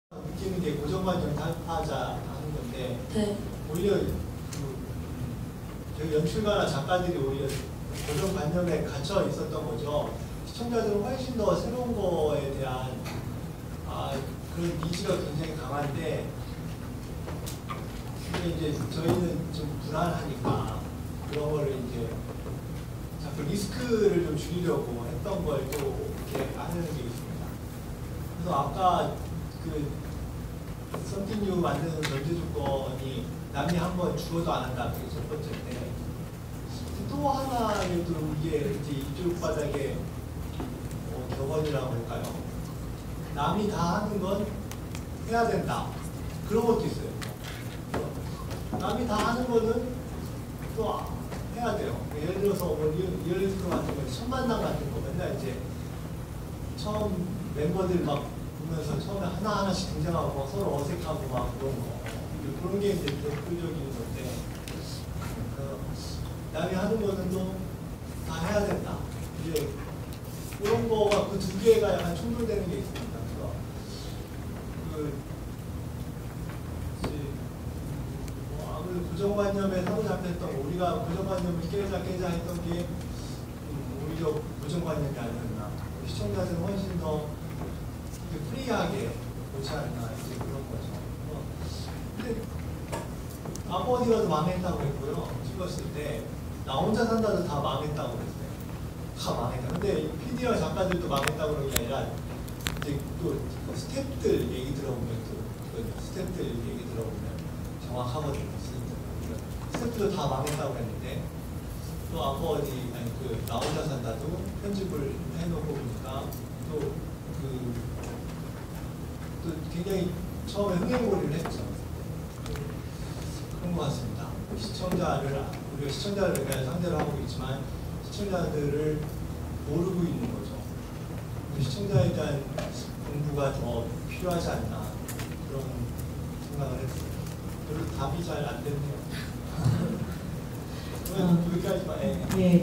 방송제작자가 갖는 고정관념이란?
[BCM Academy 2014] 권석PD가 말하는 방송관계자가 갖는 고정관념 & 방송제작자와 시청자간의 괴리.

(촬영할땐 망했다 생각했는데 대박난 예능프로그램들 있는가 에 대한 대답.)

* 권석 : MBC 예능국 부장. 무한도전 1기 '무모한도전' 런칭, 아빠어디가, 진짜사나이 기획